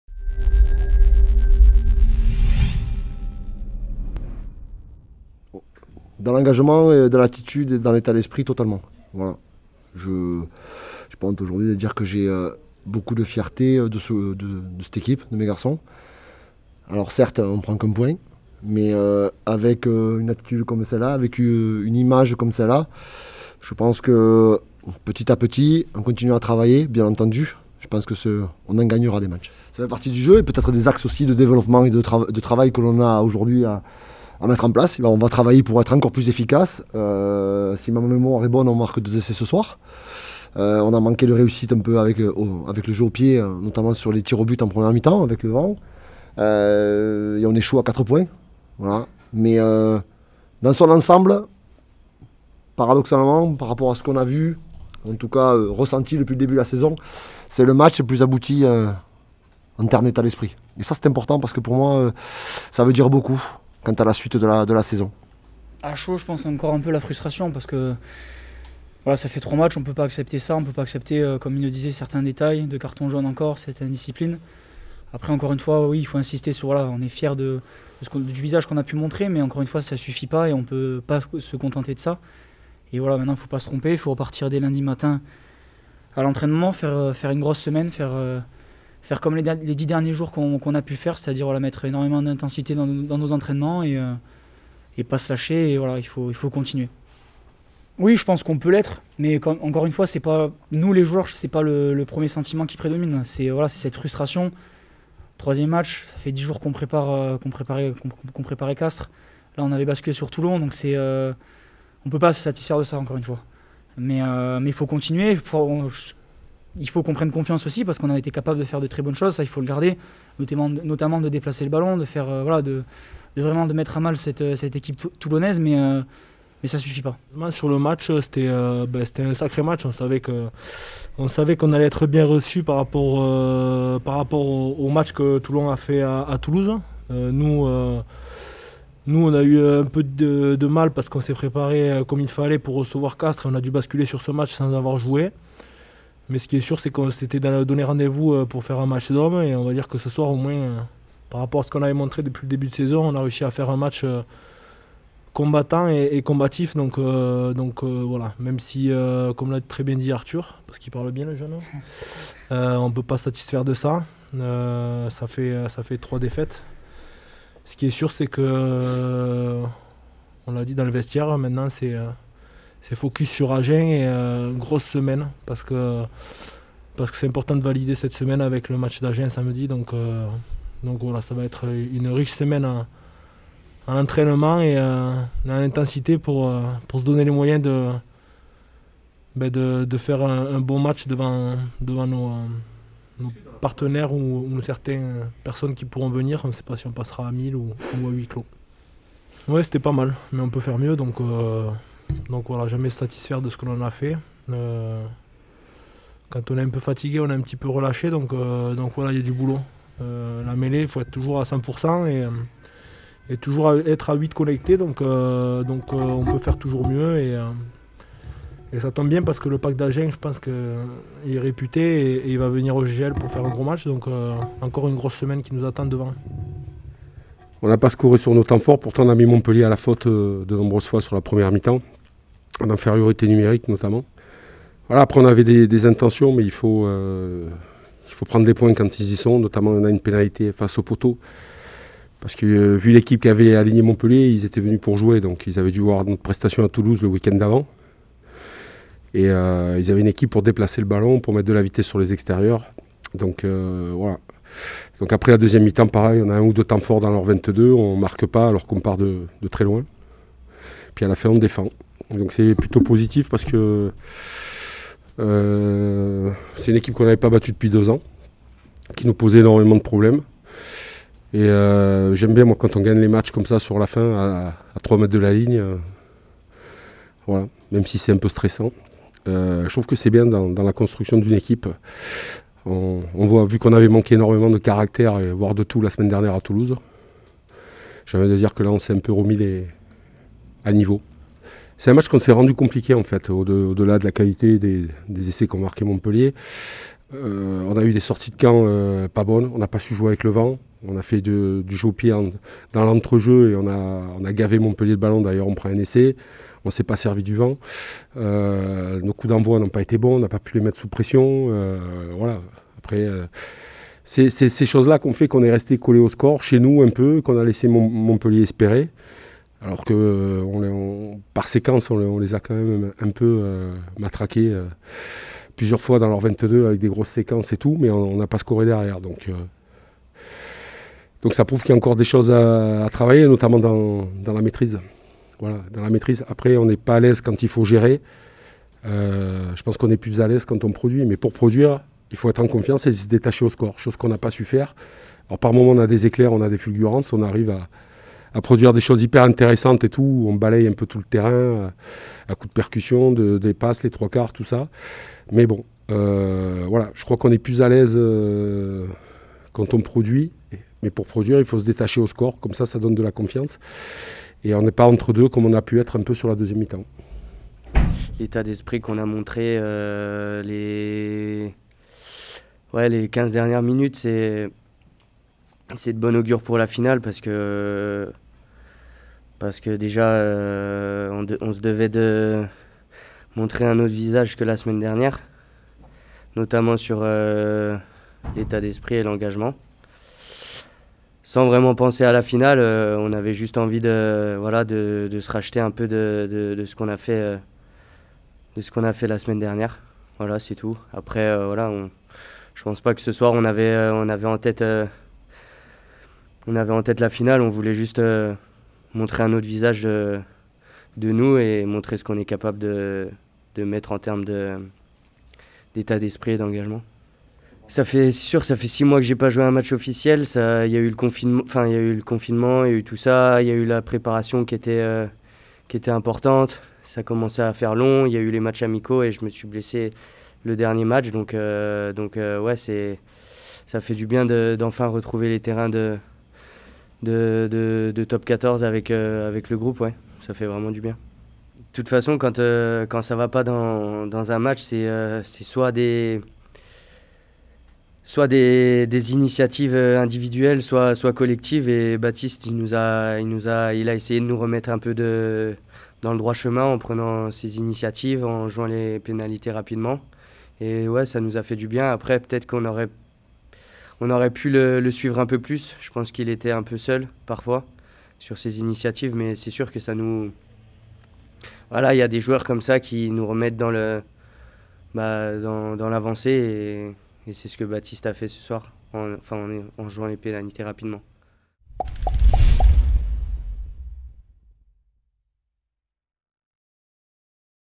Conférence de presse d'après-match J4 Toulon-Montpellier
Avec les interventions de Xavier Garbajosa, Arthur Vincentn Guilhem Guirado, Patrice Collazo et Gervais Cordin.